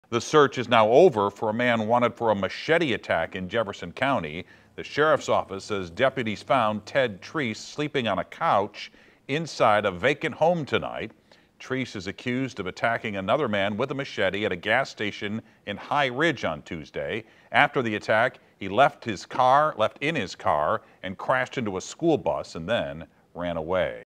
Suspect in machete assault arrested in vacant Jefferson County home
JEFFERSON COUNTY, Mo. - A man who was charged in connection with a machete assault in Jefferson County was arrested inside an abandoned building Thursday.

Ted Treece was charged with first-degree assault and armed criminal action in connection with the Tuesday afternoon incident before he was arrested Thursday night.

At around 6:45 Thursday night, deputies responded to the 10000 block of East Brook Road off Highway WW in Dittmer, Missouri, after a neighbor reported an open window at a vacant house. When deputies entered the house, they found Treece sleeping on the couch. He was taken into custody without incident.

They were able to confirm his identity by using a mobile fingerprint scan.

Grant Bissell, a spokesman for the Jefferson County Sheriff's department, said witnesses saw Treece enter a convenience store with a ski mask on and a machete in his hand at around 3:30 Tuesday afternoon. Charging documents said Treece began arguing with someone in the store and threatening the victim with the machete. He then hit the victim in the face with the machete, causing a small cut on the victim's cheek. The victim then used a glass bottle to defend himself, and Treece ran off.